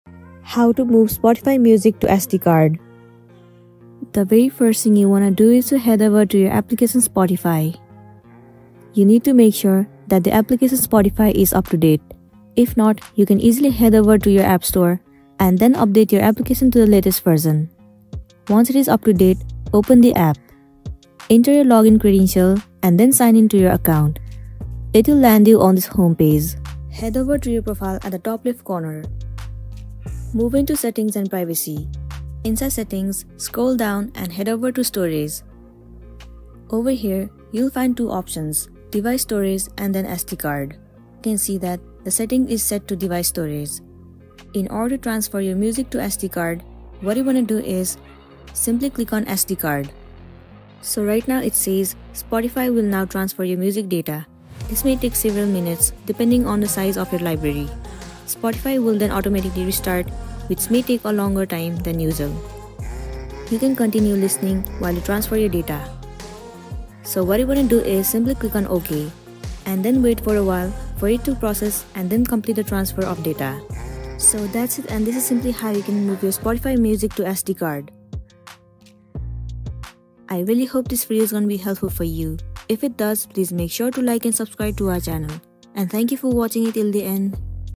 How To Move Spotify Music to SD Card [BEST Way!]
🎵📱 Move Spotify Music to SD Card (Best Way!)

Looking to save storage space on your device? This guide showcases the best method to move your Spotify music to an SD card, allowing you to free up storage while still enjoying your favorite tunes on the go.

In this tutorial:
0:00 - Introduction
0:05 - How To Move Spotify Music to SD Card (Best Way!)

Discover the most effective & real way to transfer your Spotify music to an SD card. With this method, you can easily store your music library on external storage, maximizing your device's internal storage capacity.

Save space on your device and enjoy your Spotify music collection hassle-free with our best way to move music to an SD card!